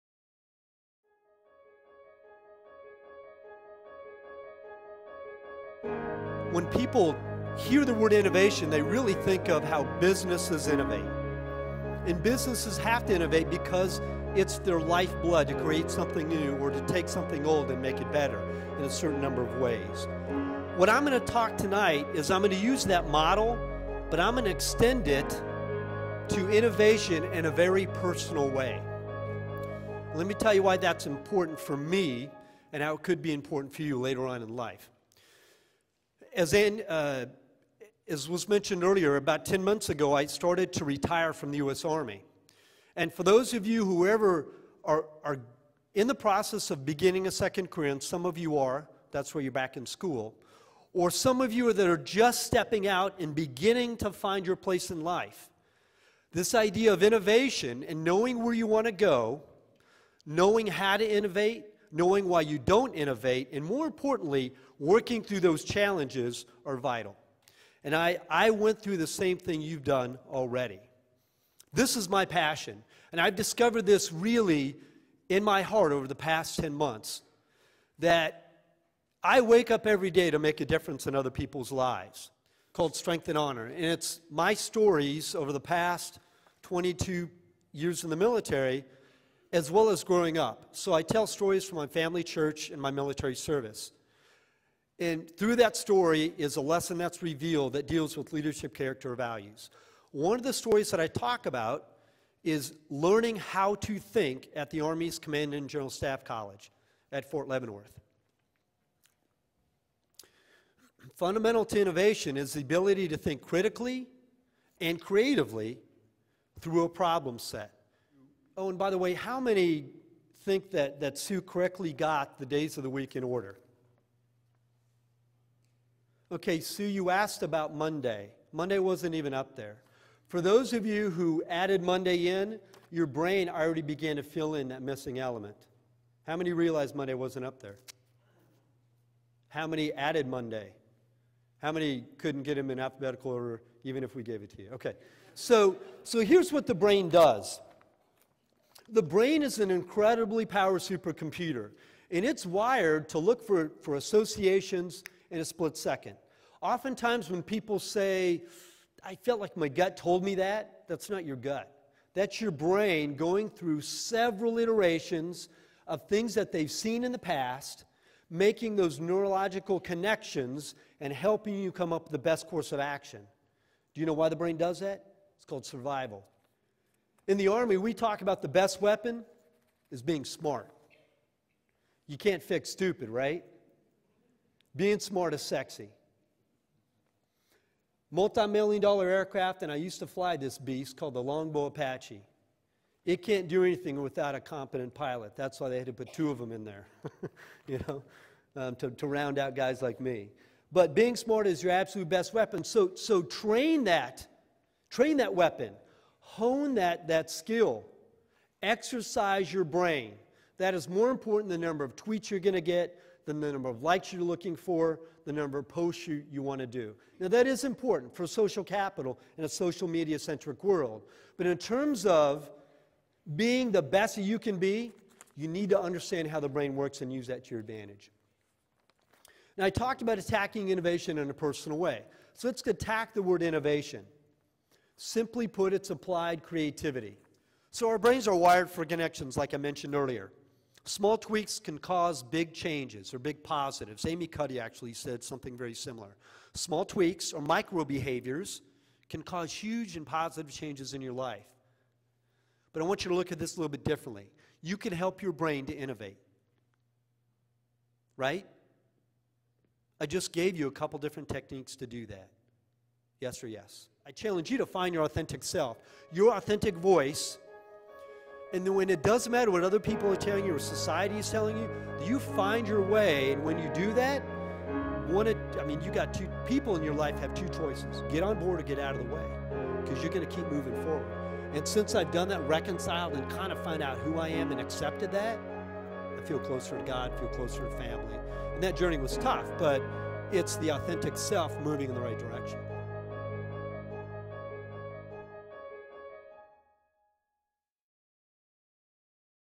John Hinck speaking on "Attaching Innovatoin"
Highlight video (5 minutes) of John speaking on "Attacking Innovation" at Iowa's 2015 Celebrate Innovation Week!